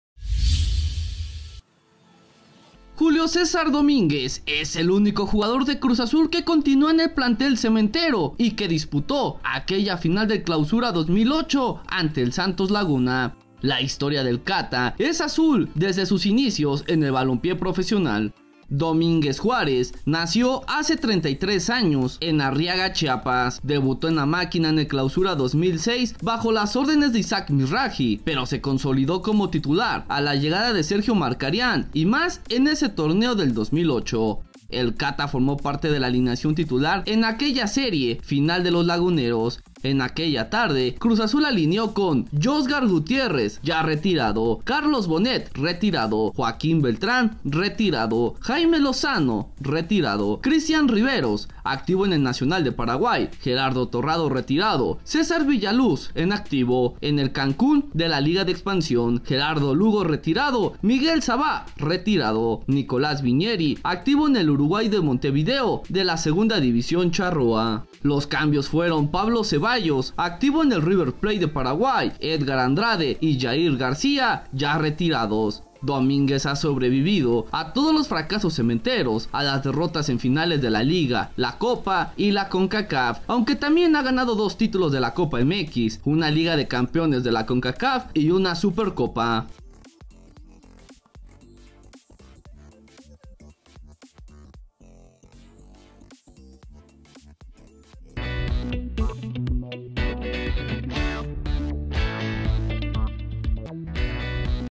Julio César 'Cata' Domínguez, el "sobreviviente" de la final del Clausura 2008
La historia del 'Cata' es azul desde sus inicios en el balompié profesional